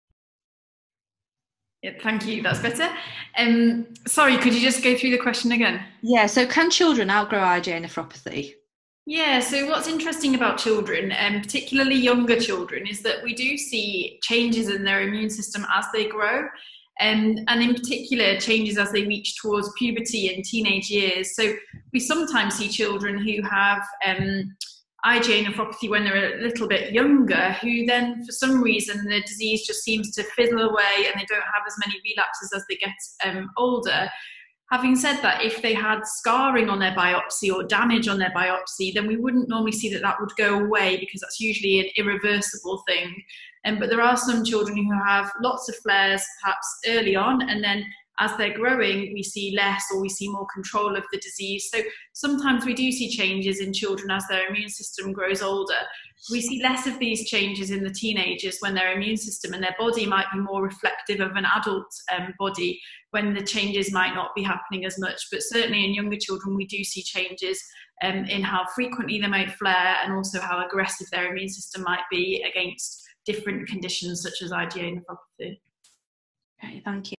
Can children outgrow IgAN? Dr Louise One
A question from the audience at the Virtual IgA Nephropathy Patient Information Event 2020. Answered by Dr Louise Oni, Paediatric Nephrologist, Alder Hey Children's Hospital, Liverpool, UK.